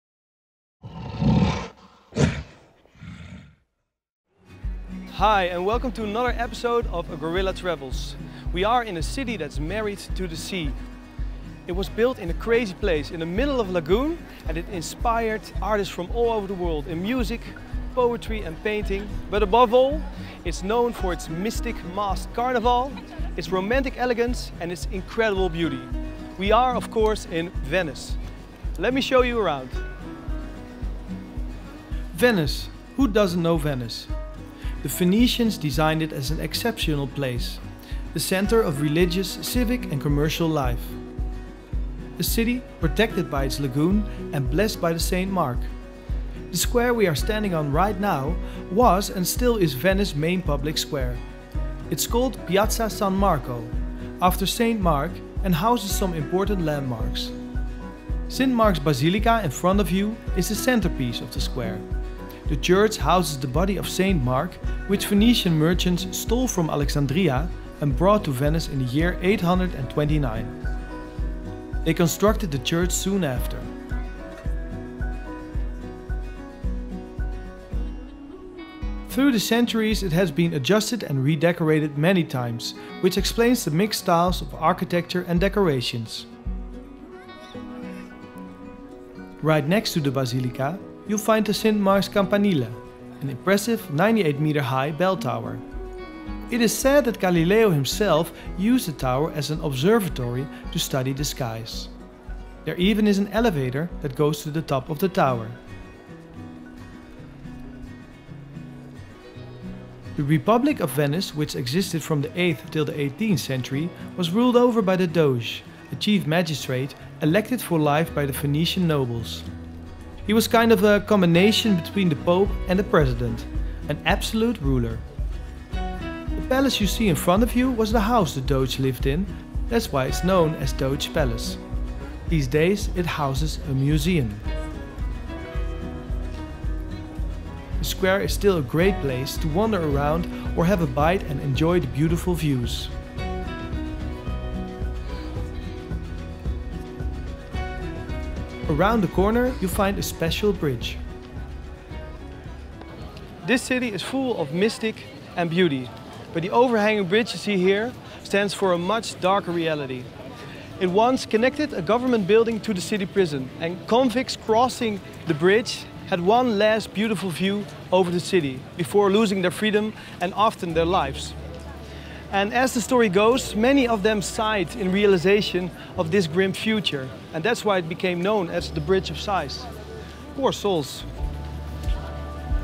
Venice, The Floating City: A Guided VR Tour - 8K 360 3D Video (short)
In this episode of 'A Gorilla Travels' we will join Peter as he explores the unique city of Venice, Italy!
See all the important highlights of the city,  including Rialto bridge, the San Marco Basilica, Teatro la Fenice and the Grand Canal of Venice.